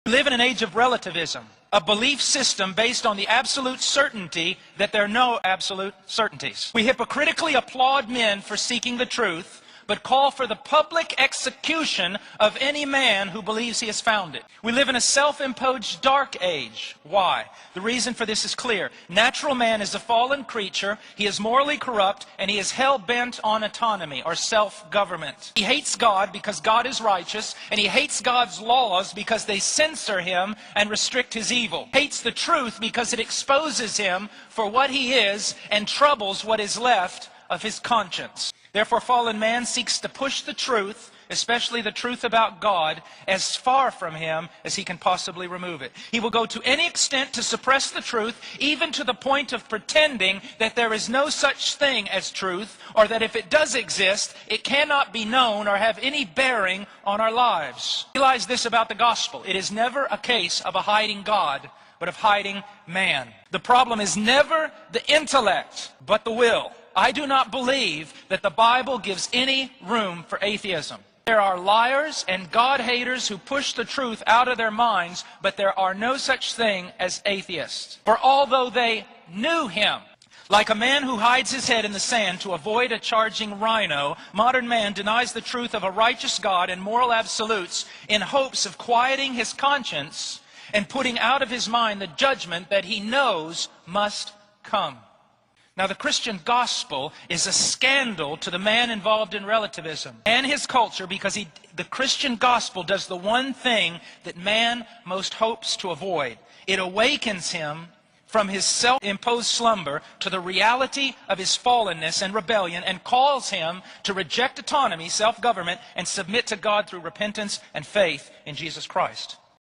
The Gospel of Jesus Christ is a Scandal to Relativism
Don't be obstinate, humble yourself before God.

Romans 1:21 For although they knew God, they did not honor Him as God or give thanks to Him, but they became futile in their thinking, and their foolish hearts were darkened.

Relativism - a belief that leads a man to utter desolation. Nobody can believe relativism in reality although they say one thing they don't follow their myth, for it is trusting in the wind which you can't see.
Man has gone hopelessly so far in relativism destroying the God-given Conscience, he became futile in his mind, a slave to his selfish evil nature & to deny his accountability to the Author of Life God

Today relativism is spreading faster than a virus in futile minds.

 What is Relativism?
A mindless concept in the culture that says there is no Absolute Truth, you have your truth & i have my truth, but if you don't agree with my truth you are canceled or called a bigot or scorn in society & the Objective Truth is now hate speech.

The Word of God is Absolute Authority
We as creatures ought to submit to the Creator's Word.
1 John 4:6 We are from God, and whoever knows God listens to us, but whoever is not from God does not listen to us. This is how we recognize the Spirit of truth and the spirit of falsehood.

Christian En Armatura Kadosh Theos